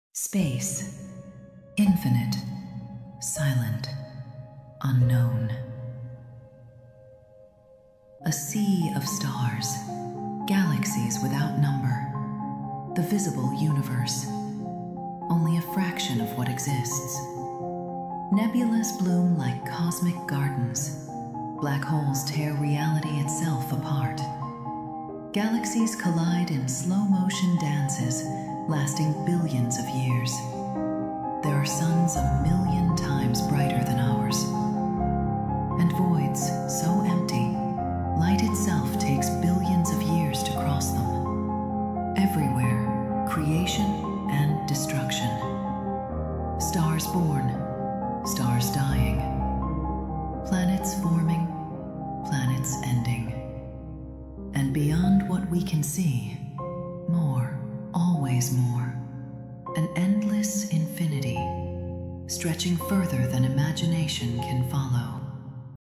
“The Silent Beauty of Space: Stars, Galaxies & Mysteries Beyond Imagination 🌌✨”
Step into the silent beauty of space — a universe filled with glowing stars, endless galaxies, and cosmic mysteries we still can’t explain. From the birth of stars to the vast unknown stretching beyond our vision, this documentary-style video takes you on a breathtaking journey through the universe.

✨ What you’ll see in this video:
– Stunning cosmic visuals
– Deep-space galaxies and nebulae
– The science behind stars and the universe
– The mysteries that continue to puzzle astronomers

If you love space, science, or visually immersive explorations, this video is for you.
Sit back, relax, and let the universe speak in silence. 🌌Documentary